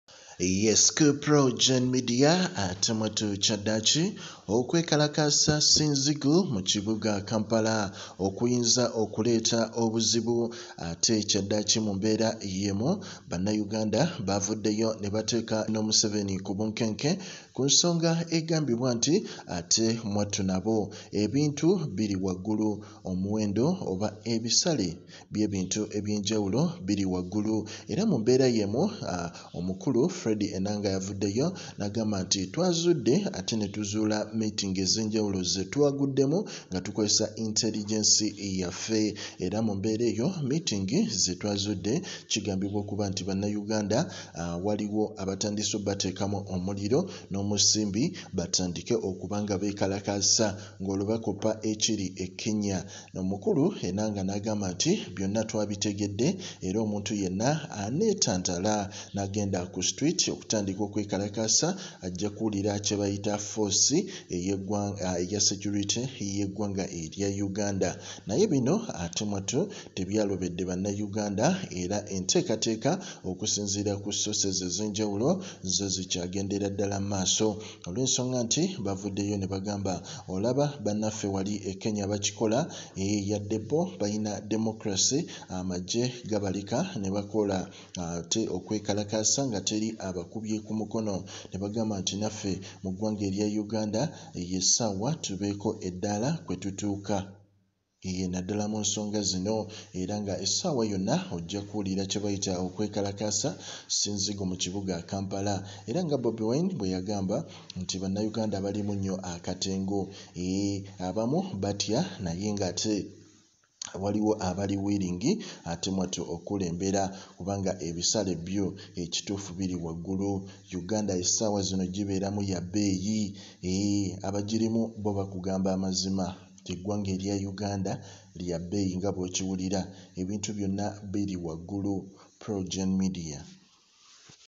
Okwekalakaasa okujjako museveni ngabwekili ekenya Amajje ne Poliisi gavudeyo negalabula abanetantala
Museveni gamumyuuse Okwekalakaasa kutabuse Bobi wine Akakasiza Ekiddako kubakungu ababba amabaati
Just learnt of the violent arrest of these comrades of the Torture Survivors Movement UG who protested against the corruption and shameless theft of iron sheets belonging to the people of Karamoja. Those who stole the iron sheets are moving in convoys paid for by the taxpayer and getting VIP treatment, while those who are calling for their prosecution are the ones being prosecuted and persecuted. This is what happens when you have a thieving criminal regime in charge of the affairs of a nation. The humiliation will come to an end! Uganda will be free.